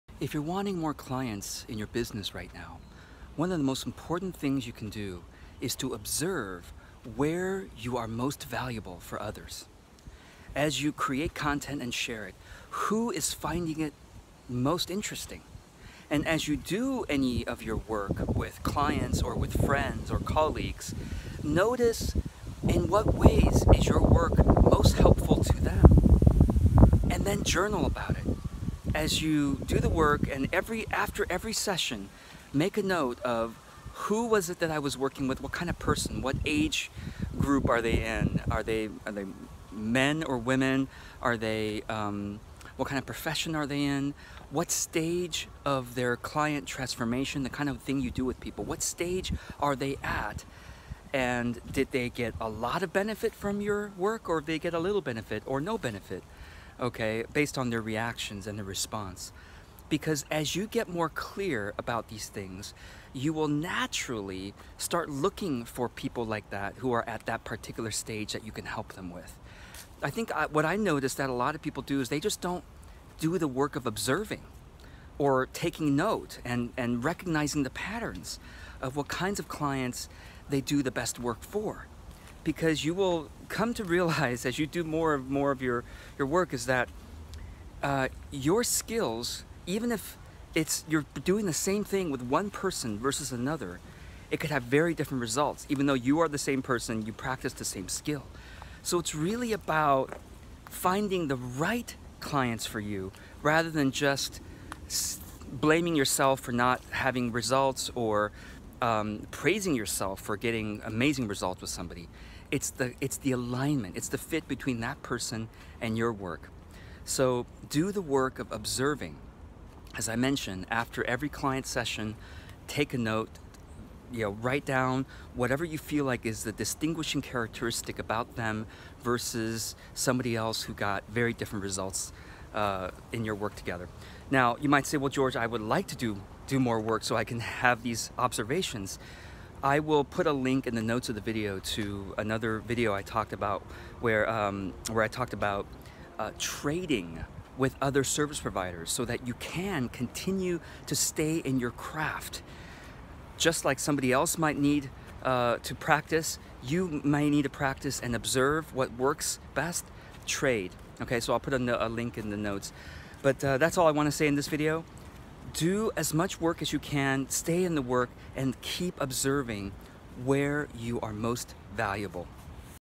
Observe what you do that your clients most appreciate
How to describe what you do? Who are your ideal clients? Take notes after client sessions.

If you’re trying to sell a service, for example coaching or healing, you might hear from marketing experts to never sell the modality but always sell the benefits and always speak to the ideal client.

But you’re left wondering — “how do I know which benefits people really care about… and what type of person is my ideal client?”

To answer these questions, you need to:

Be taking notes about client sessions as you have them.
Work with different types of people, to have more individuals experience your modality, so you can see for whom your skills are most valuable and in what situations.
Let me talk about each of these.

Doing More of Your Work
If you haven’t done your work for many different types of people, you are limiting yourself. You don’t know how your method/modality/process affects different people.

There’s a “secret” that experienced service providers know: the more ideal the client is, the better their results.
In other words, it’s not just about you improving your skills. Even with the same skills, one client of yours will get dramatically better results, and another client gets very little results.

The determining factor? The kind of client you’re working with.

In the beginning, when you simply need work, you’re happy to accept any person who wants to be your client. That’s good, because you then get to experience a variety of clients.

If you are looking to do more of your work, try trading with other service providers for feedback.

But it is equally important to take notes after each client session for the purpose of understanding where your skills are most valuable, and to whom.

Taking Notes
“The palest ink is better than the strongest memory.” (Chinese proverb)
When trying to describe your service, or the benefits of what you provide, or who your ideal client is, do you just “go by intuition” or “try to remember”?

It’s much more accurate, and easier, when you have notes to look at, so you can see the actual patterns and commonalities between your best clients (those who get the most results with you)— and your “worst” clients (those who get the least results.)

Get into the habit of taking notes after each client session:
What success or benefits have they experienced, since the time of the previous session they had with you? (If this is the first session, skip to question 3.)
About this benefit/success, what did you do that helped?
For the session you just completed: What problem did they come to the session with?
What was the “aha! moment” or the biggest benefit that the client experienced?
What did you do that helped them the most? What exercise, process, or idea did you share with them?
If you answer the above questions right after each client session, and do this on a consistent basis, you will start to notice patterns. And it will help you describe your services more accurately and to write more authentic marketing copy.

Another benefit: you’ll be getting new and relevant ideas for your content.

If you haven’t been doing this, I do not recommend taking notes for previous sessions if it’s been a long time, or if you have multiple clients, as it can all meld together in your mind. Every time we access a particular memory, it actually changes. You might be making things up.

Just start today with your note-taking. The sooner the better.

Who are your ideal clients?
In addition to post-session notes, it’s also helpful to take overall notes about each client.

When the client first starts to work with you:
Demographics e.g. their age, gender, profession, field of study (helpful if you run FB ads) — you can find this info on their Facebook or Linkedin profile.
How they found you. Be specific: which post or ad did they see (if they remember)? Which friend or colleague referred them to you? (Reach out with gratitude to that referral source.) What were they searching for online that allowed them to come across your site?
What they’re looking for. What motivated them to seek out your services? What problem or goal or situation?
What they’ve already tried. What solution or service have they already tried, before working with you? What hasn’t worked well for them, and why? (This helps you to see what gaps are in the market, that your service can fill, that you can emphasize in your marketing.)
When the client completes a package of sessions with you: